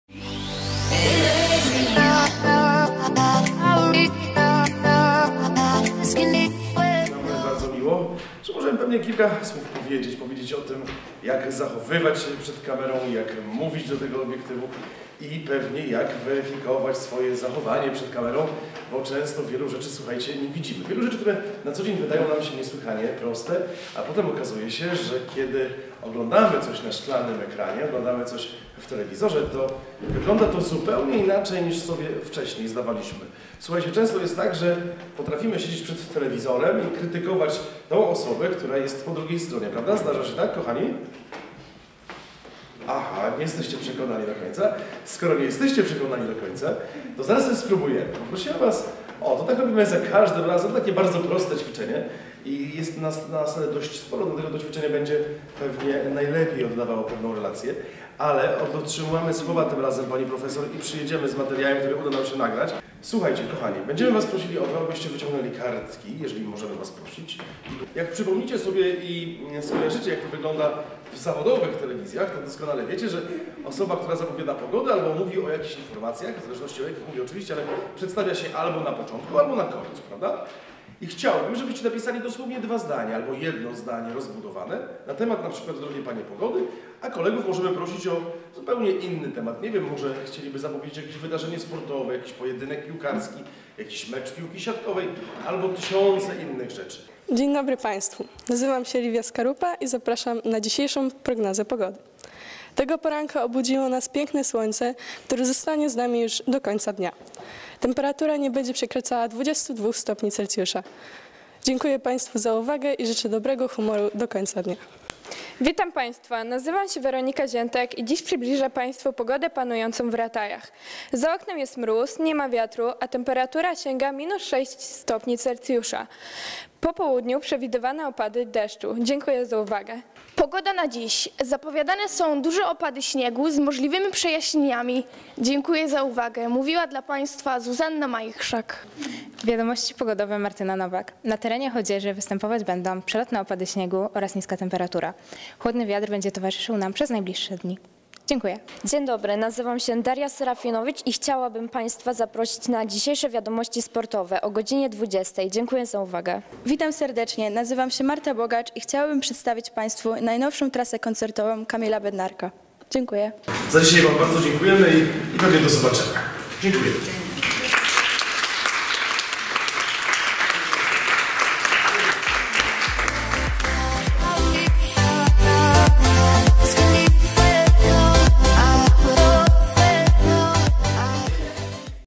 Warsztaty telewizyjne - skrót
Skrót z warsztatów zachowania się przed kamerą w ZS Rataje prowadzone przez Chodzieską Telewizję Kablową Antserwis.